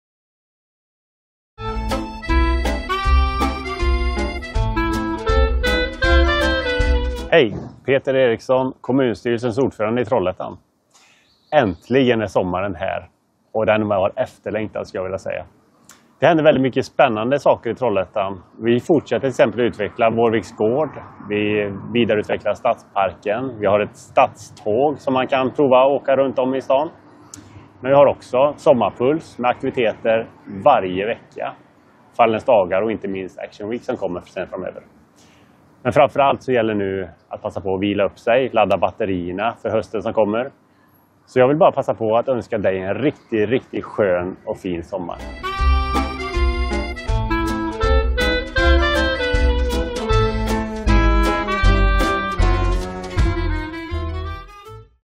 En sommarhälsning från Peter Eriksson (M) Trollhättan - LTVU.SE
Trollhättans Stads kommunstyrelses ordförande Peter Eriksson (M) ger en
sommarhälsning 2024 till kollegor och innevånare.